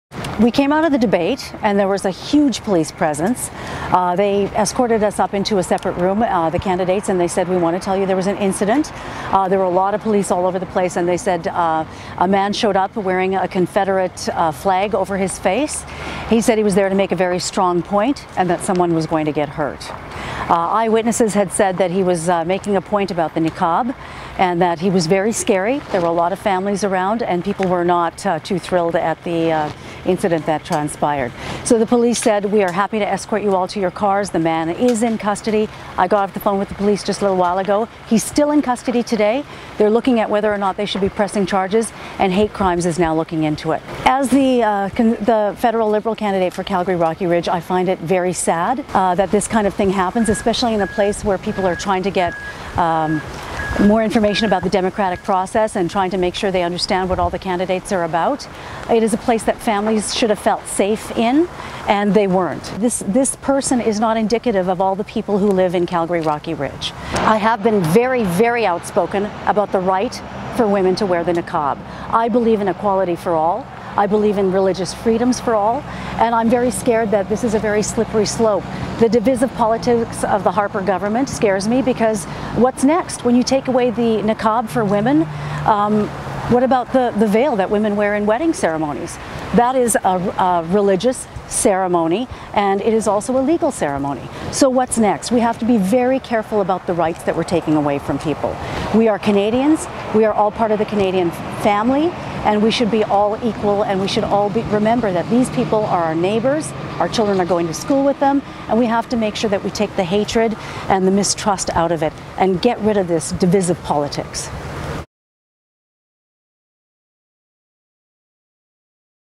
All-candidate debate disrupted by Confederate flag-wearing man
Liberal candidate for Calgary Rocky Ridge Nirmala Naidoo addresses a disruptive and threatening incident by a man wearing a Confederate flag over his face at an all-candidates debate last night.